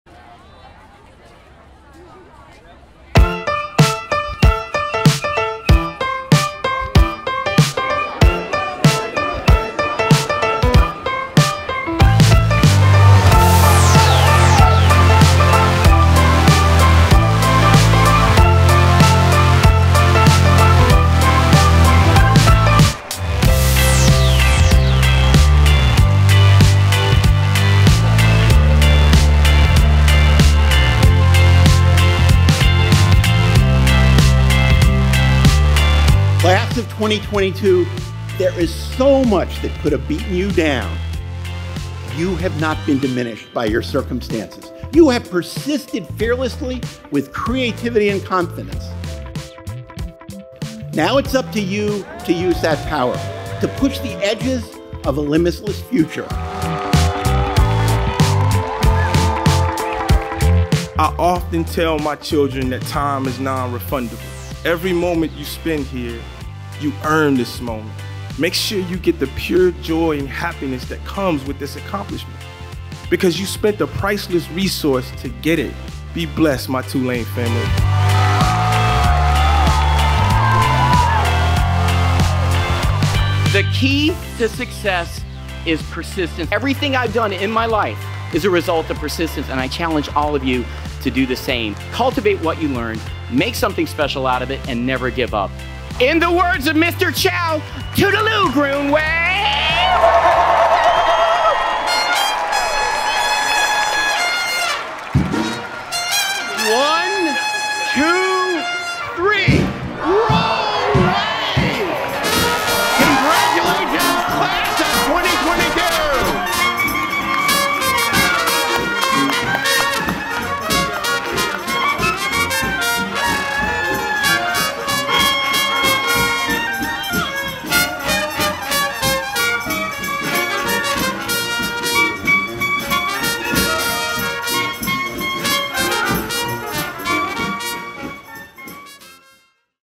2022 Tulane University Unified Commencement Ceremony
The Tulane class of 2022 celebrated their academic achievements on May 21, 2022, at the unified commencement ceremony in Yulman Stadium. Surrounded by friends and family, graduates experienced a uniquely-Tulane celebration to send them off in style.

Actor, producer, writer, and doctor Ken Jeong was the keynote speaker, giving graduates advice to persist through the toughest of challenges.

President Mike Fitts bestowed honorary degrees on Dr. Jeong, Tulane alumnus Gen. David H. Berger, and posthumously to Henry "Hank" Aaron, accepted by his wife Mrs. Billye Aaron on his behalf.

Student speaker Dr. Russell Ledet encouraged his fellow graduates to celebrate their accomplishments.

Dr. Michael White and the Original Liberty Jazz Orchestra provided a celebratory soundtrack for the event.

For more information on Tulane commencements, visit commencement.tulane.edu.